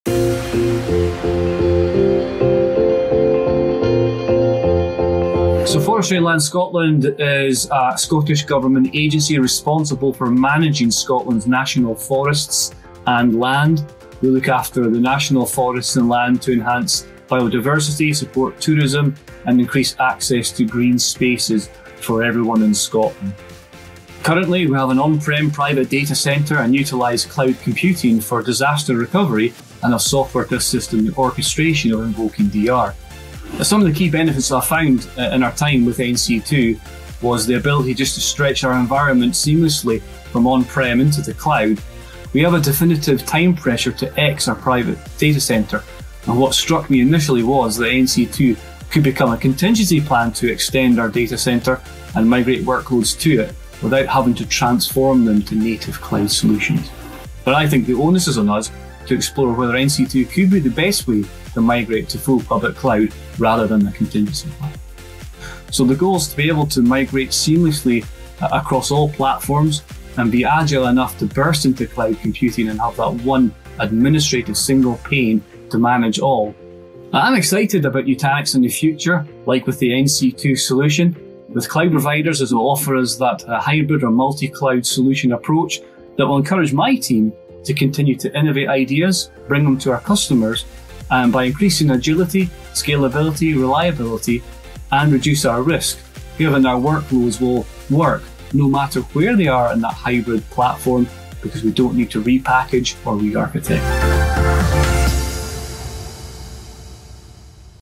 How to Avoid Application Repackaging or Re-Architecting with Nutanix NC2 | Nutanix Customer Stories
Forestry and Land, Scotland leverage Nutanix NC2 hybrid cloud to avoid application repackaging or re-architecting.

Nick Mahlitz, Senior Digital Infrastructure Manager of Forestry and Land Scotland, discusses his experience with Nutanix hybrid cloud and how Nutanix Cloud Clusters (NC2) on Microsoft Azure helps to seamlessly stretch their existing data center to cloud for simple migrations without requiring application transformation.

👉 Check It Out Take Nutanix for a no-strings-attached test drive!